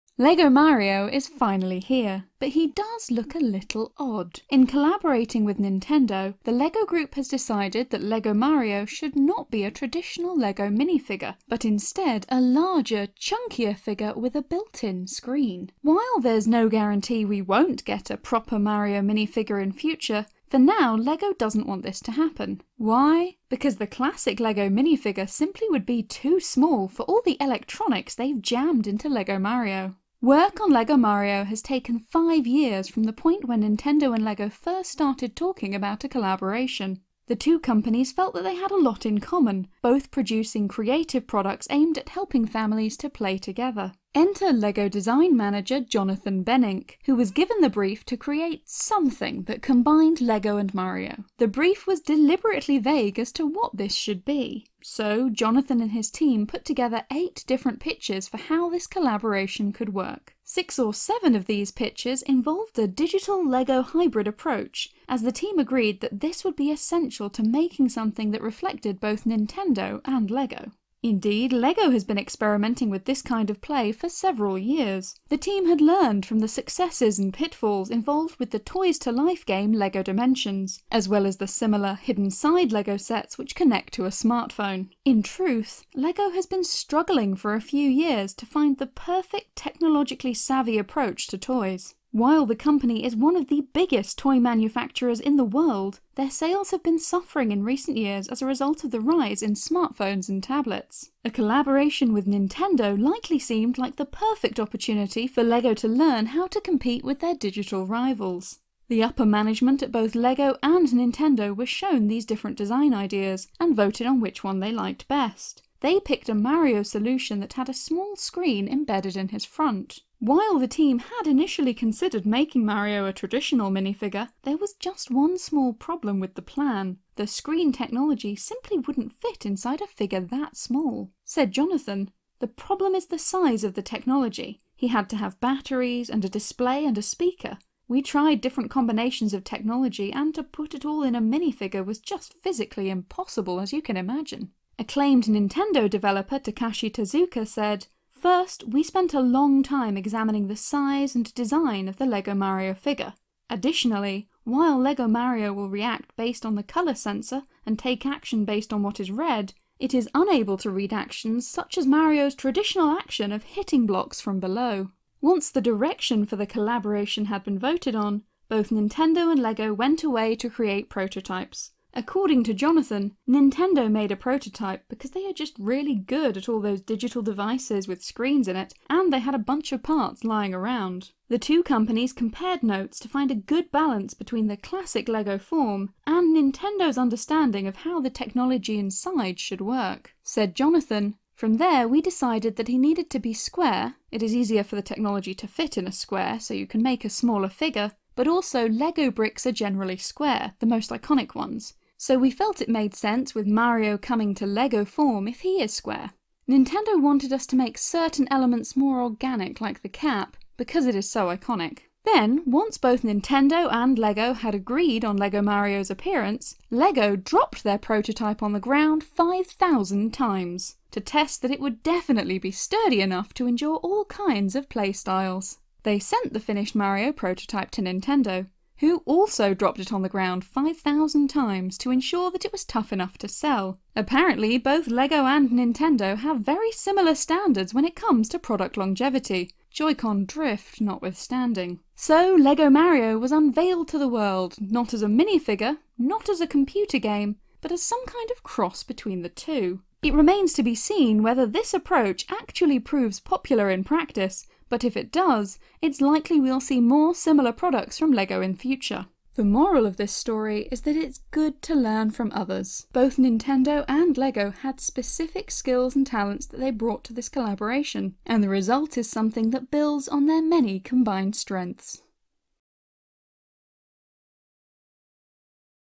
Why Lego Mario Looks So Weird
Mario Lego is here! But why isn’t doesn’t it have minifigures?

The collaboration between Lego and Nintendo is certainly interesting. Instead of a traditional Lego set with classic minifigs, Mario is a large, chonky boi with a screen built into his abdomen like a Teletubby.

Lego Mario somehow interacts with elements in his “level” (the other bricks in the set” using NFC technology. We’re honestly not too clear on how it actually works, and whether it’s any more enjoyable than “hurr hurr Lego goes bleep bloop”. Fingers crossed it doesn’t end up feeling like a bit waste of time and money!

Interestingly, Nintendo and Lego both worked on this together, each building their own prototypes and choosing to stress test Lego Mario using their own internal departments. Nintendo supplied the technical knowhow, Lego supplied the colourful blocks.

It’s rare for a piece of Nintendo merchandise, or a licensed Lego set, to feature such an intense collaboration. Whether or not Lego Mario ends up being, y’know, good, the process by which it was created is simply fascinating.

Of course, some of us would prefer a more traditional Lego set with minifigures for Mario, Luigi, and most definitely Princess Peach. Maybe we’ll get them some time later down the road…

Like, seriously, it’s weird. Lego Piranha Plant and Lego Bowser Jr exist before Lego Princess Peach. Lego Thwomp AND Lego Whomp, but no Lego Luigi. Major oversight.

Anyway, hope you’re staying safe, and if you’re playing with Mario Lego, here’s hoping you’re enjoying it!